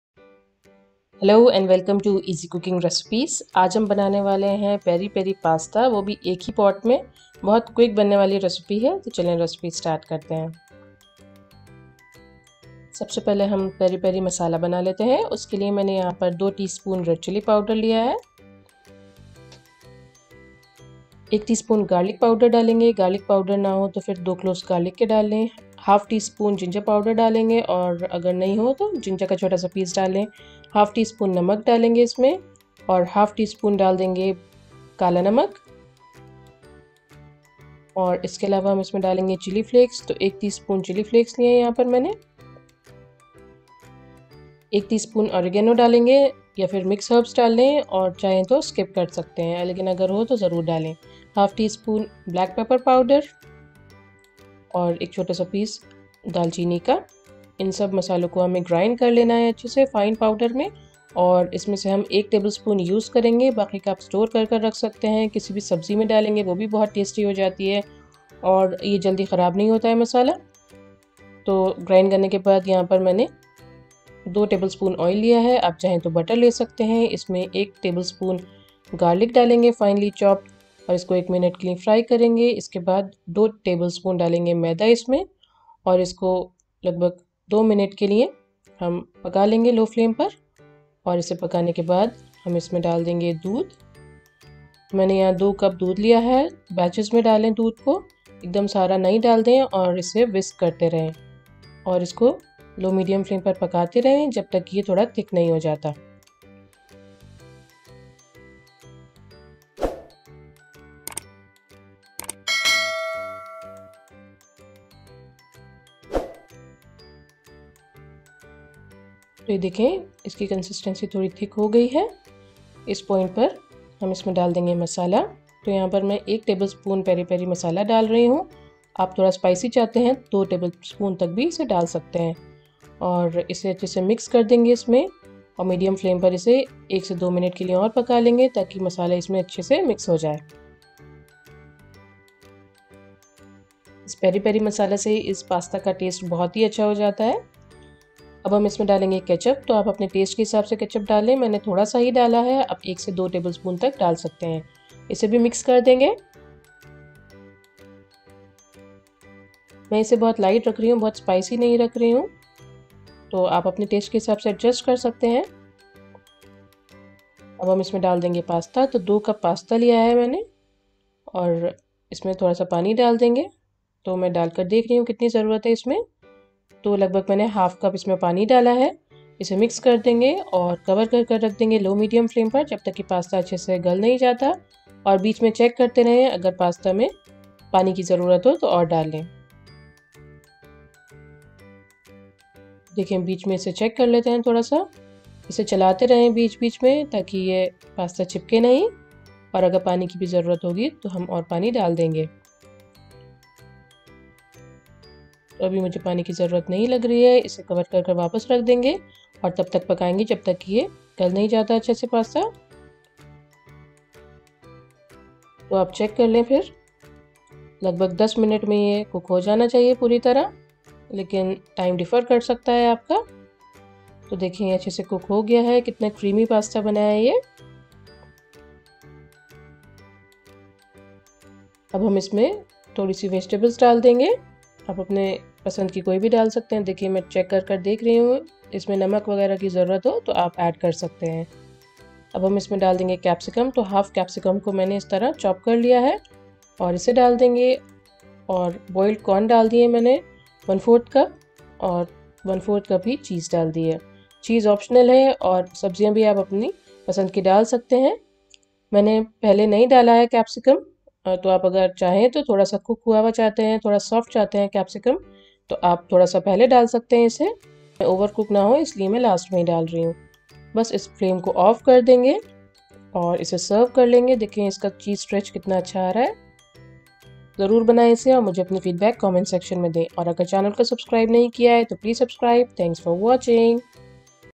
One Pot Peri Peri Pasta | Easy Tasty Pasta Recipe | Easy Cooking Recipes
INGREDIENTS
Peri peri masala;-
2 tsp red chilli powder
1 tsp Red Chili Flakes
1 tsp Oregano or Mixed Herbs Seasoning
1 tsp Garlic Powder
1/2 tsp ginger powder
1/2 tsp salt
1/2 tsp black
 A small piece of cinnamon
1/4 tsp black pepper powder or some black pepper corns
2 cups of pasta
1 tsp salt
2 tsp oil/butter
1 tbsp finely chopped garlic
1/2 capsicum roughly
 1/4 cup boiled corns
2 tbsp all purpose flour
2 cups of milk
1-2 tbsp Tomato Ketchup
2-3 tbsp Mozzarella Cheese, (optional)